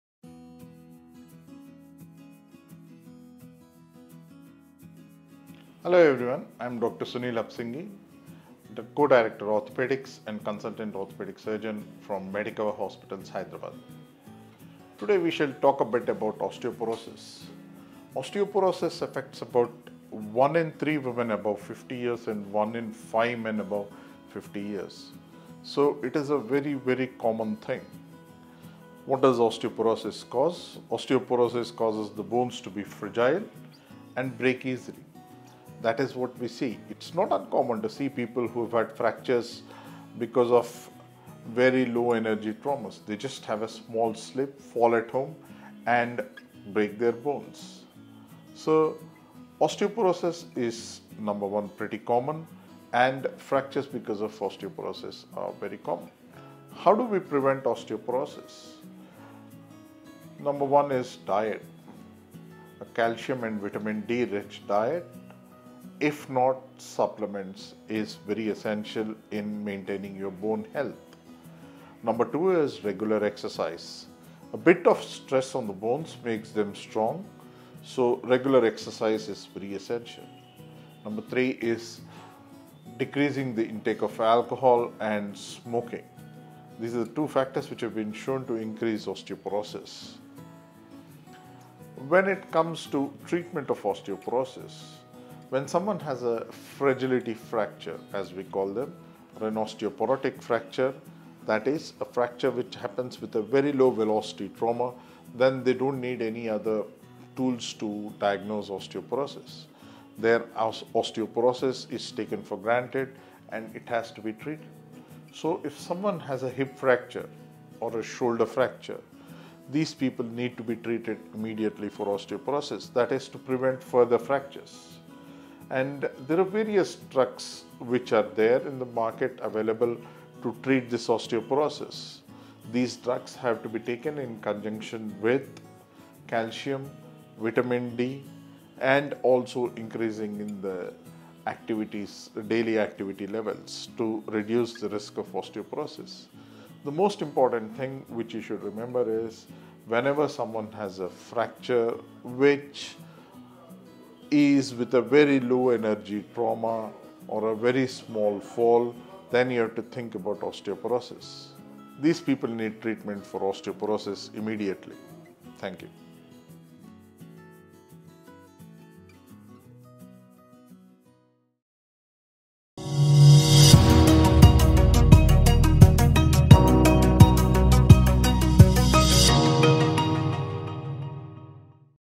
What Is Osteoporosis | Causes Of Osteoporosis| Prevention Of Osteoporosis | TimesXP Health
There are several causes of Osteoporosis. Here in this video our health expert
Dr Sunil Apsingi shares in detail on causes of Osteoporosis and how it can be prevented. Check out the video.

Dr Sunil Apsingi,
Co-Director Orthopedic Surgeon,
Medicover Hospitals